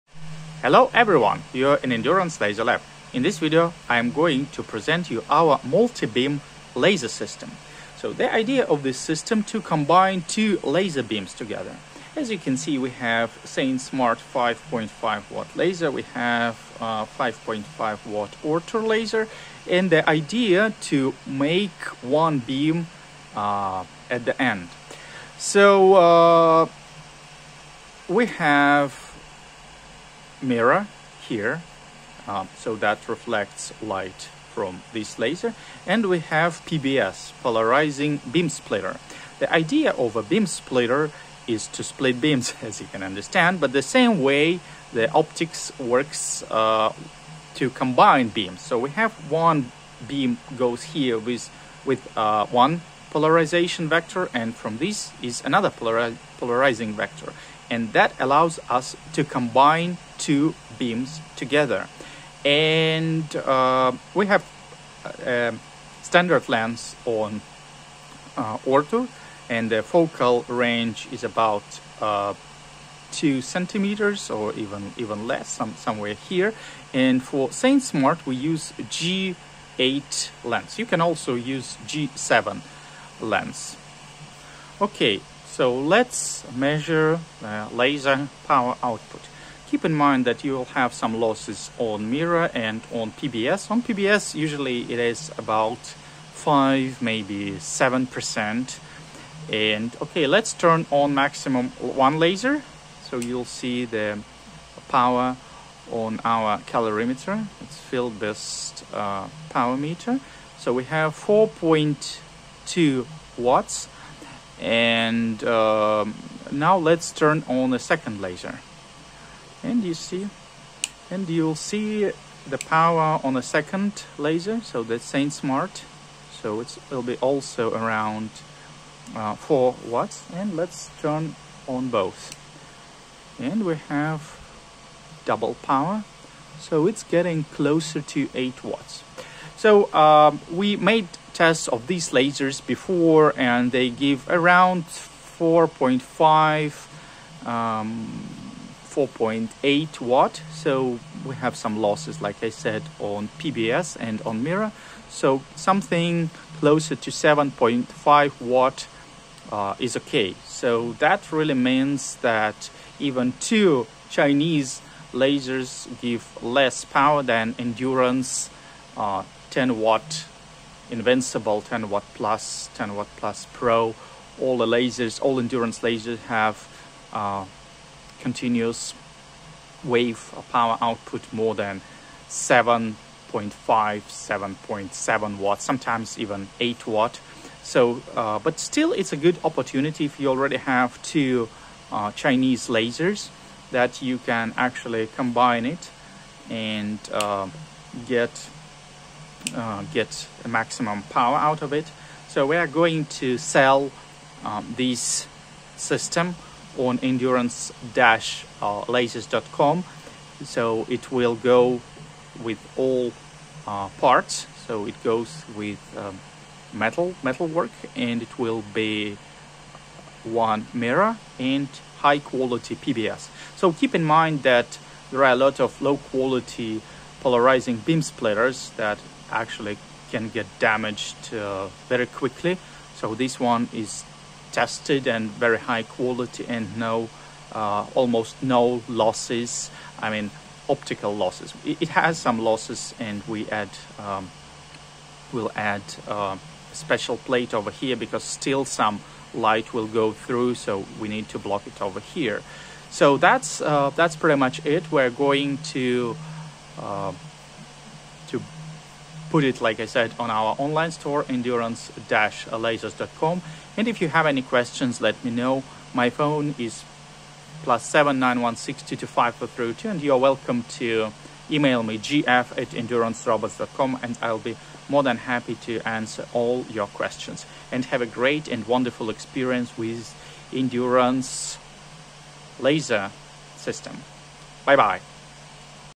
An Endurance laser beam combiner system. Combines 2 beams in 1 for any diode laser
A presentation of an Endurance laser beam combiner system.

Allows combining 2 lasers beam with different polarization vectors.